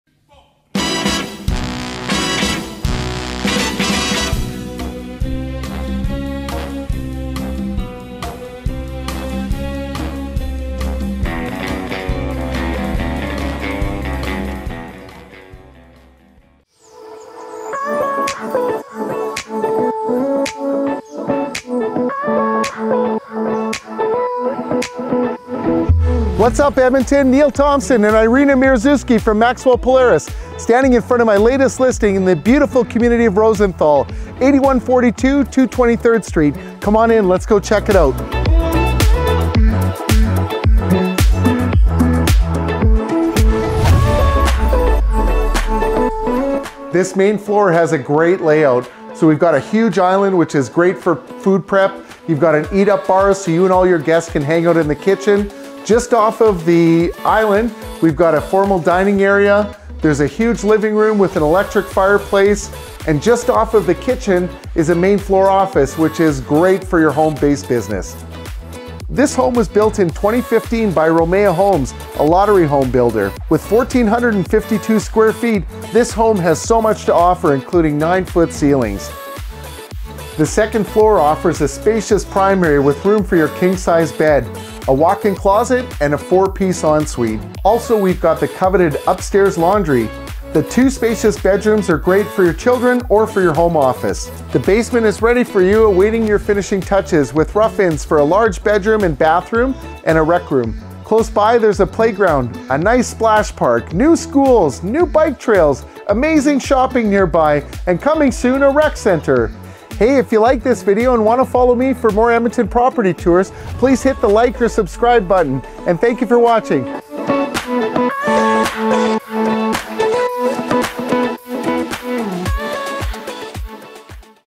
Edmonton Property Tours in Rosenthal with Neil Thompson, Edmonton Realtor. 8142 223 Street NW
Welcome home to 8142-223 street located in the community of Rosenthal! This amazing duplex built in 2015 by lottery home builder Romaya Homes! This home has so much to offer with 3 great sized bedrooms + main floor den, Double detached garage - what else do you need to raise young family in sought after community of Rosenthal! Main floor offers hardwood floors, the stylish kitchen has granite counter tops, flushmount sink, premium SS appliances and corner pantry! Main floor Den is there for your office or kids area! Upstairs you will find 3 good sized bedrooms and coveted upstairs laundry! The primary is spacious and has a walk in closet with closet organizers and a spa like ensuite! The West sunny back yard is great for your family gatherings as well as kids play area and of course double detached garage for all your toys! Rosenthal is a family oriented community with a playground, spray park, bike trails/walking paths and new schools for kids of all ages! New rec centre is planned to be in the area in near future. Great shopping and West Edmonton Mall are nearby, and A. Henday and Whitemud for quick access.